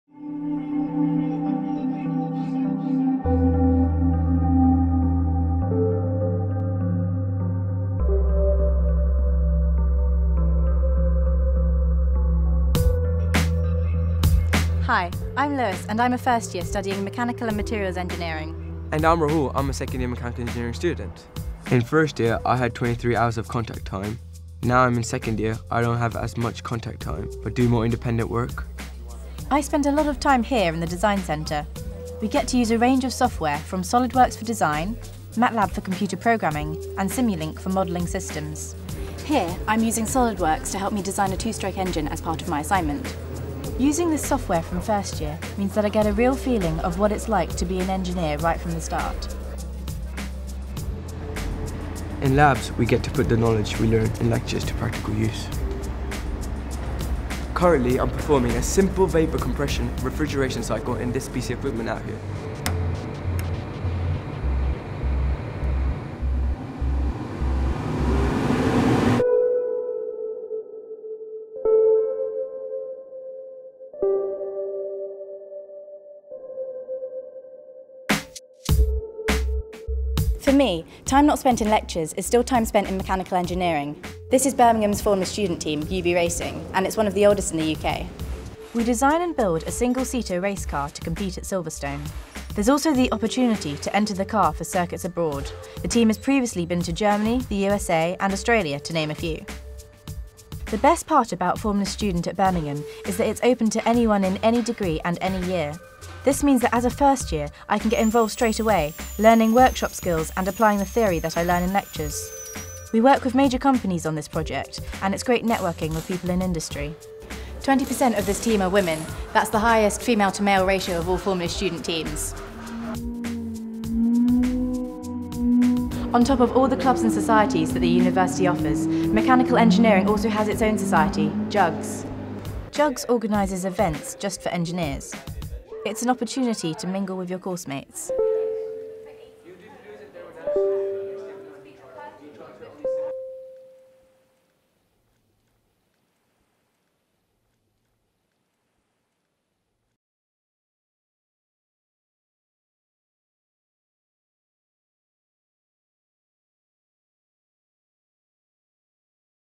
A Day in the Life of a Mechanical Engineering Student
One of several "Day in the Life" films Creative Media has completed over the past 12 months, this film showcases the day to day activities of a mechanical engineering student at the University of Birmingham.